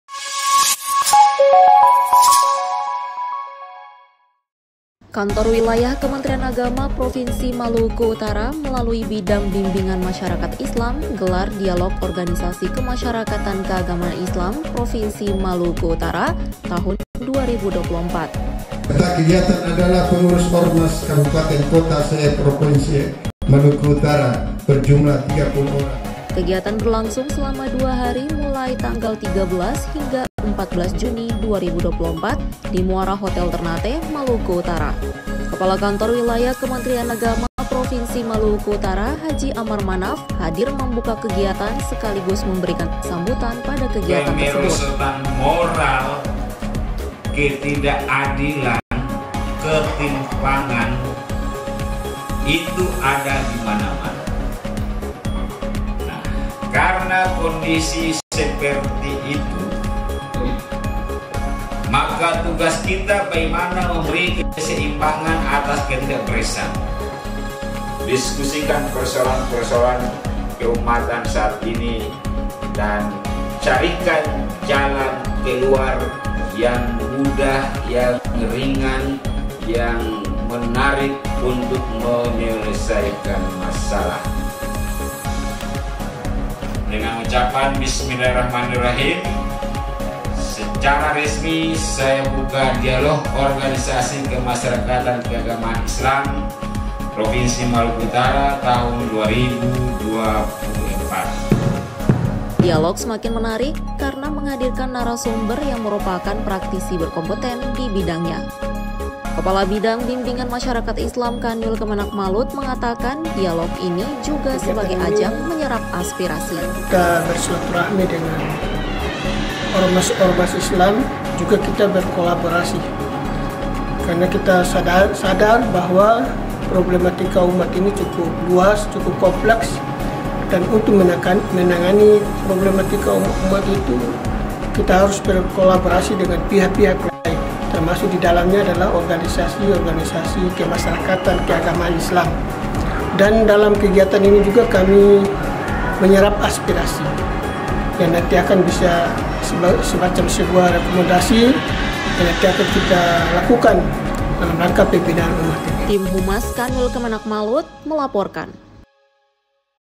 DIALOG ORMAS KEAGAMAAN ISLAM PROVINSI MALUKU UTARA 2024
Kantor Wilayah Kementerian Agama Provinsi Maluku Utara melalui Bidang Bimbingan Masyarakat Islam gelar Dialog Organisasi Masyarakat (Ormas) Keagamaan Islam Provinsi Maluku Utara 2024. Kegiatan berlangsung mulai 13-14 Juni 2024 di Muara Hotel, Ternate, Maluku Utara.